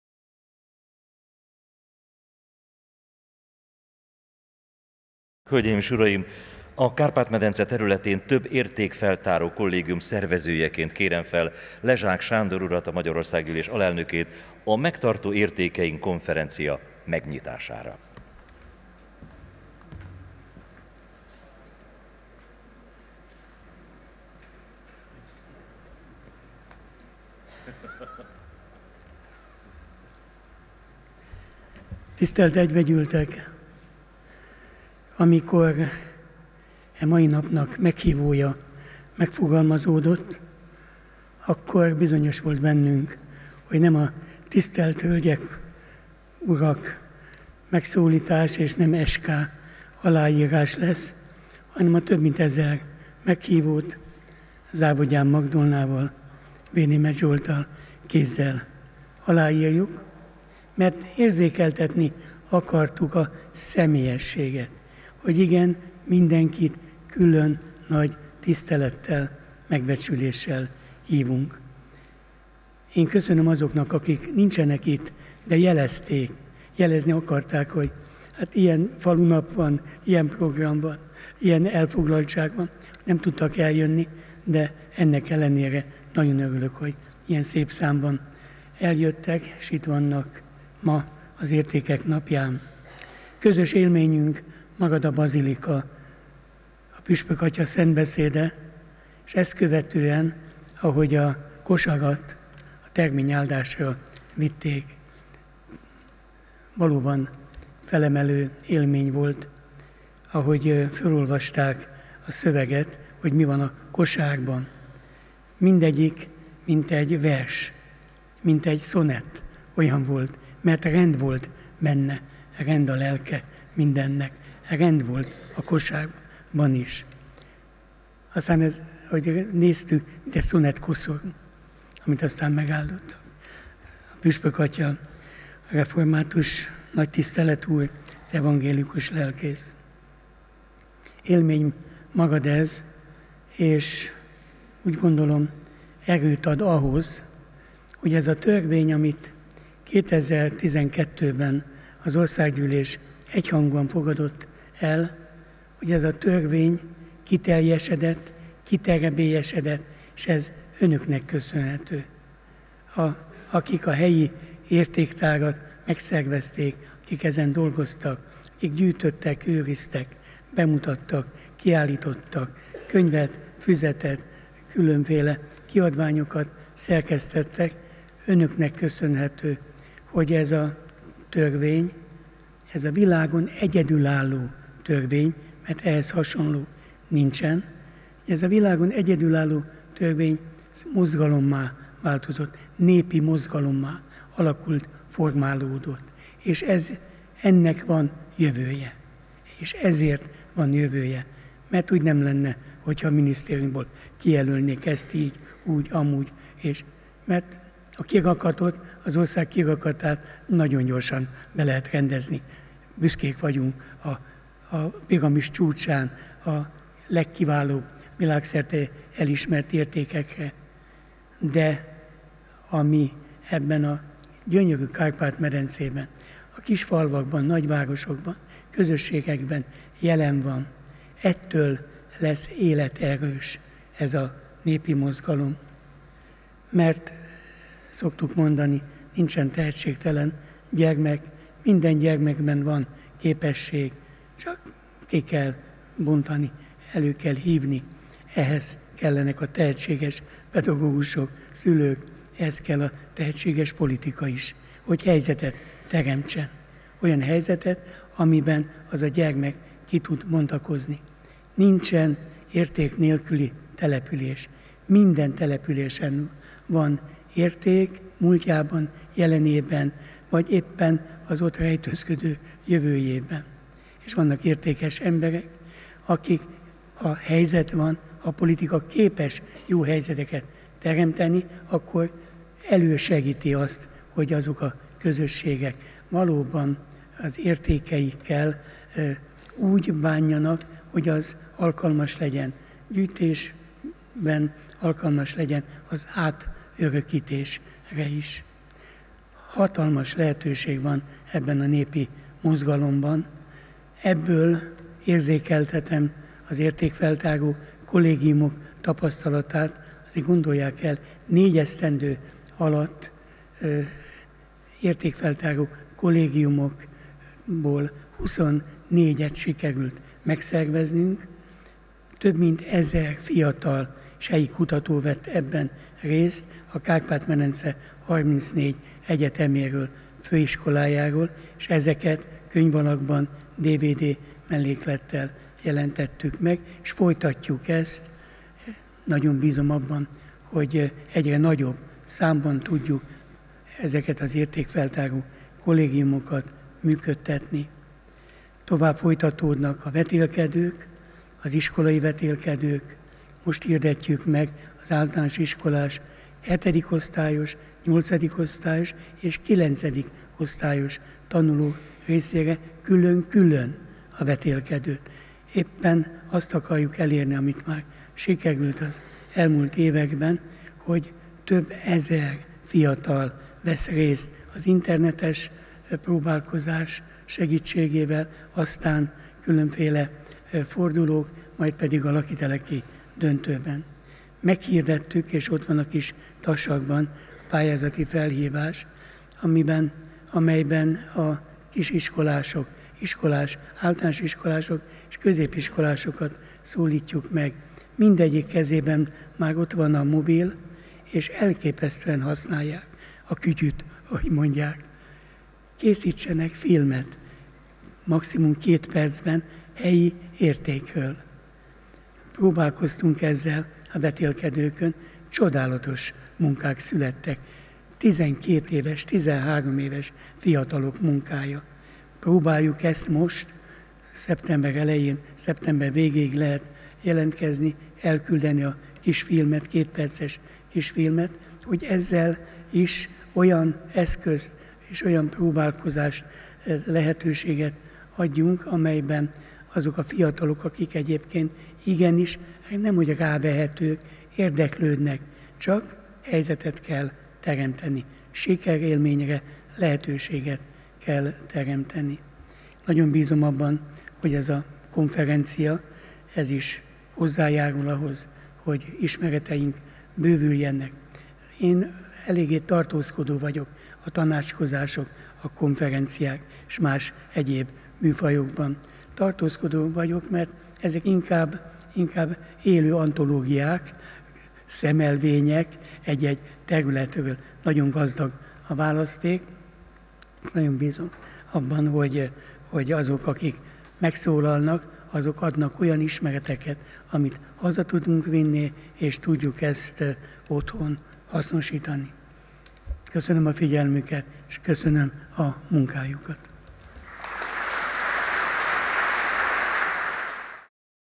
"Nincsen érték nélküli település" / Lezsák Sándor
Lezsák Sándor, az Országgyűlés alelnöke, a Hungarikum Bizottság tagja.
IV. Magyar Értékek Napja 2019.szeptember 7.